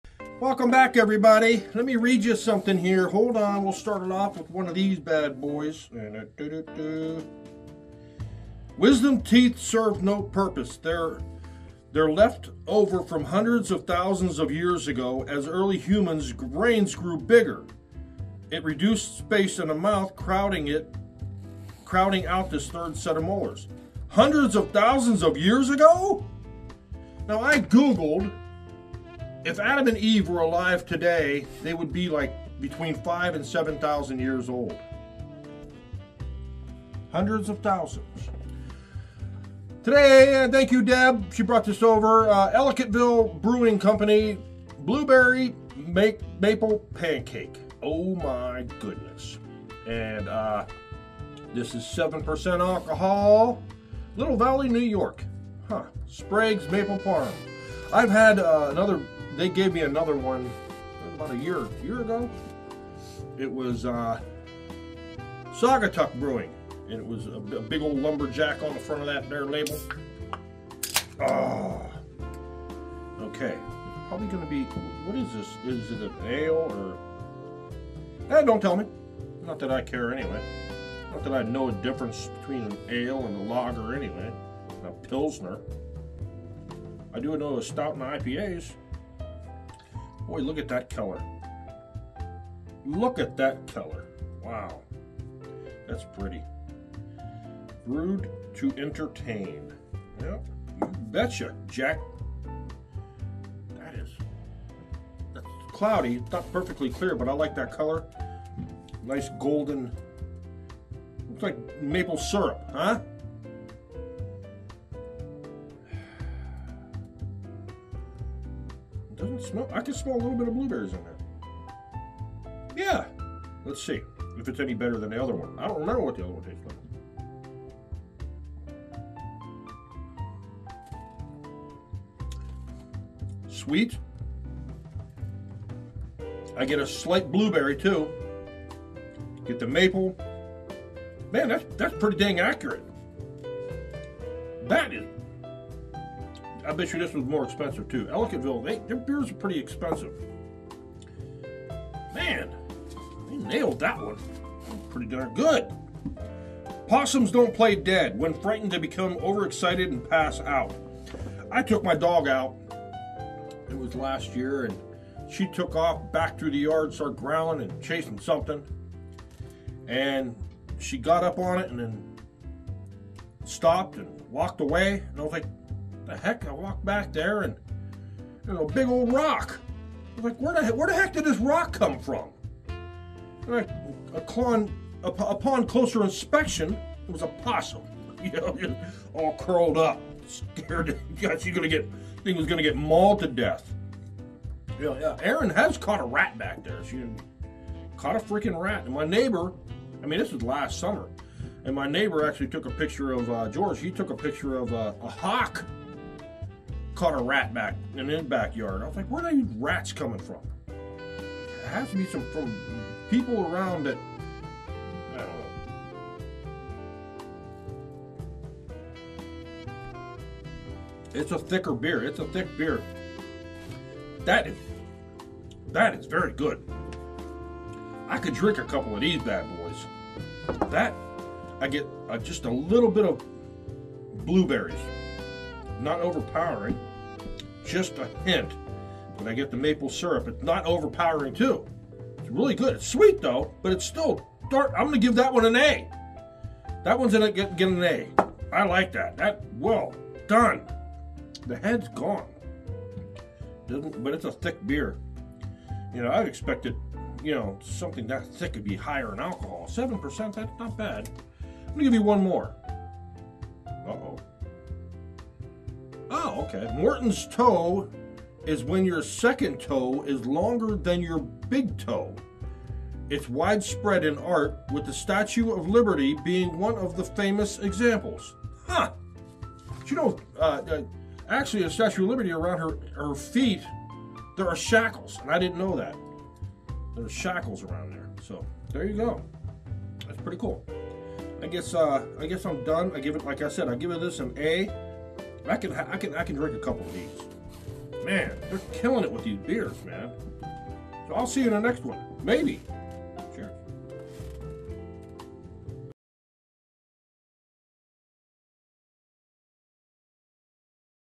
Ellicottville Brewing Blueberry Maple Pancake
Just a regular guy trying different beers telling you if I like them or not, and a couple fun facts thrown in as well. Enjoy.